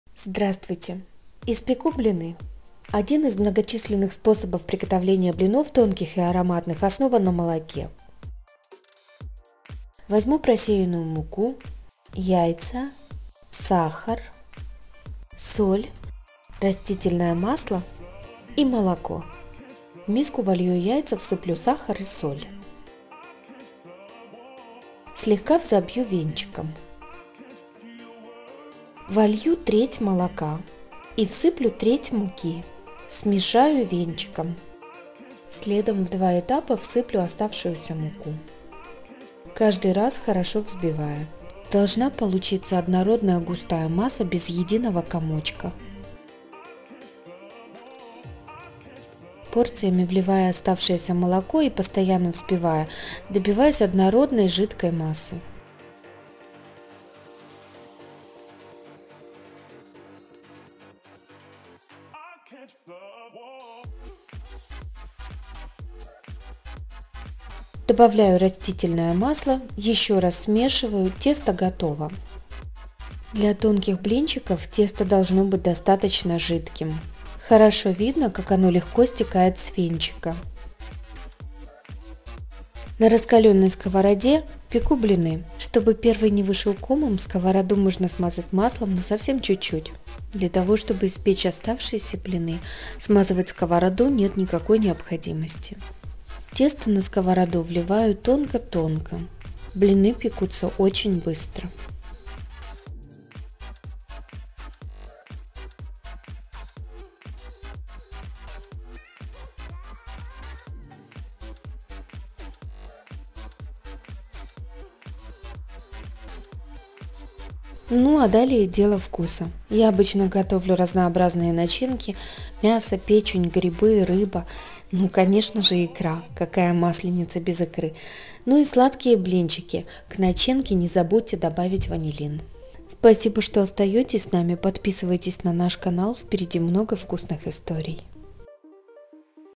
Блины на молоке. Нежные блины на молоке. Рецепт вкусных блинов.
Готовлю блины на молоке. Блинчики, пожалуй, одно из самых популярных блюд, а разновидности блинов, есть практически в каждой кухне, где используют муку. Разнообразие используемых начинок, способно удовлетворить самые взыскательные вкусы.

Блины на молоке:

- 400-450г муки
- 2 яйца
- 50-75г сахара
- 15г соли
- 75мл растительного масла
- 1л молока